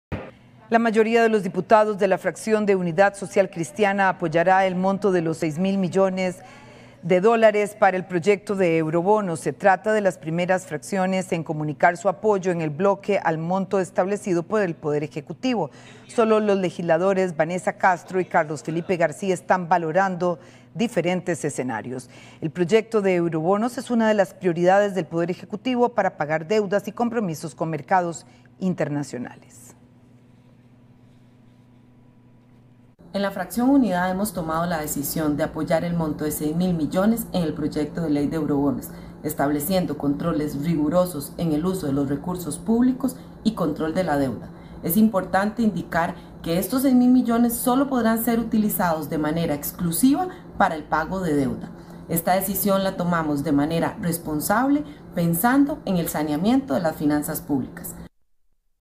Avanza el tema de los eurobonos en la Asamblea
Diputados de Unidad Social Cristiana apoyarían, en su mayoría el apoyo del monto de 6 mil millones de colones.
Este proyecto sería de vital importancia para el Poder Ejecutivo.